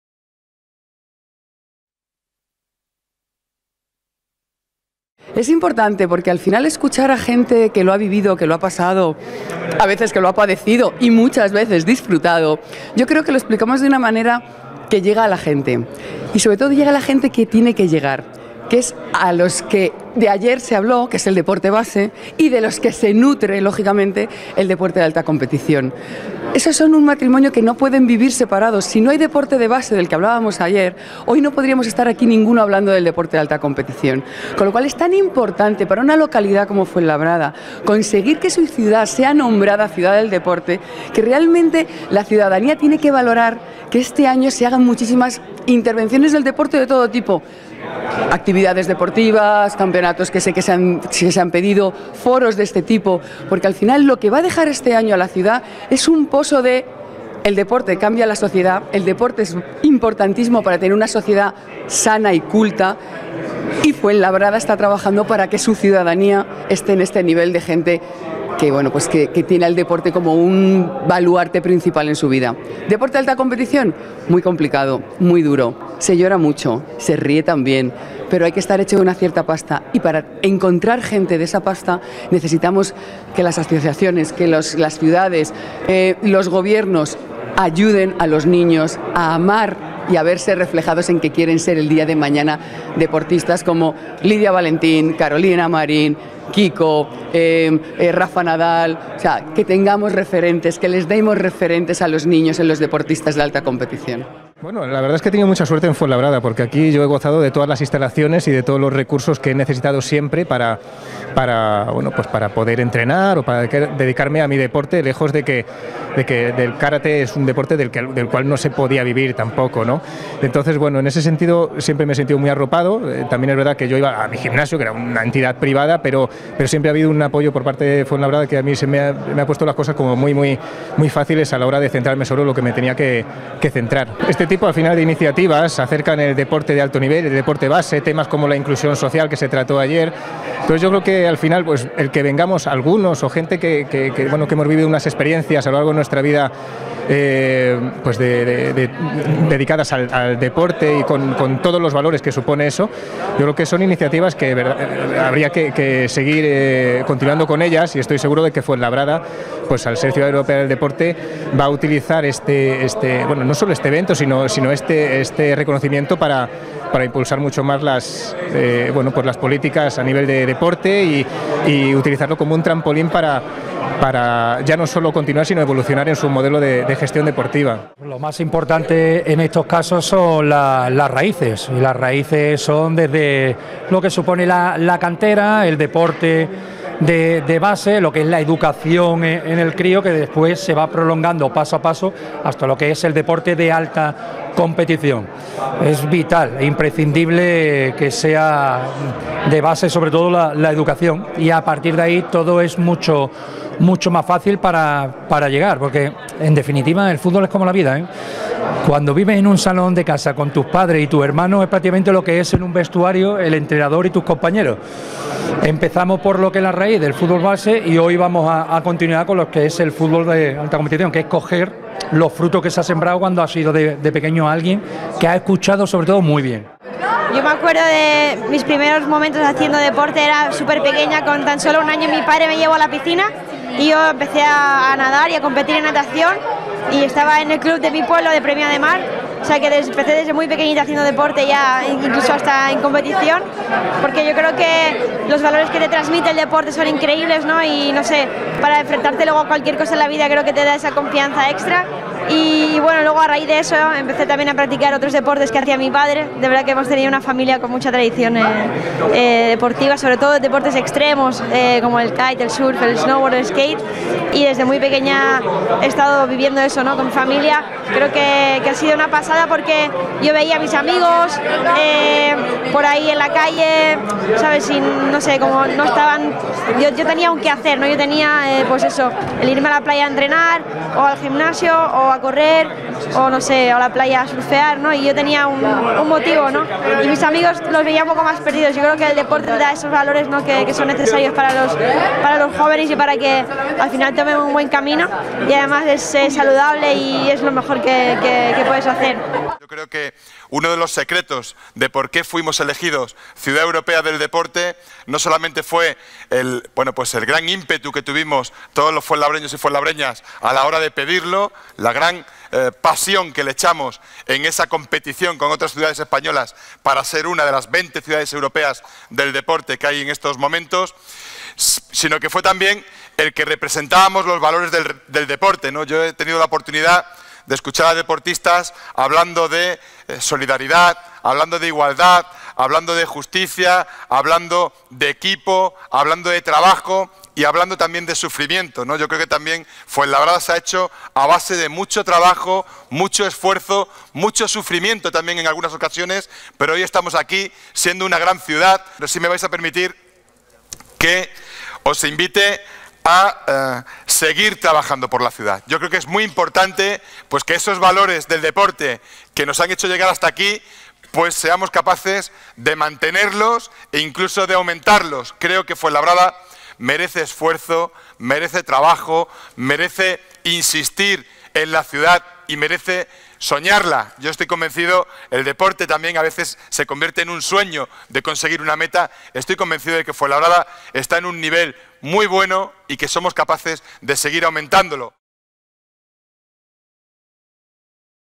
Clausura del Foro de las Ciudades 2019
Segunda mesa y clausura del Foro de las Ciudades 2019. Con las intervenciones de Kiko Narvaez, Gisela Pulido, Fernando San Jose, Coral Bistuer, Side Lowe y John Swanson.
Clausura por parte del alcalde de Fuenlabrada, Javier Ayala.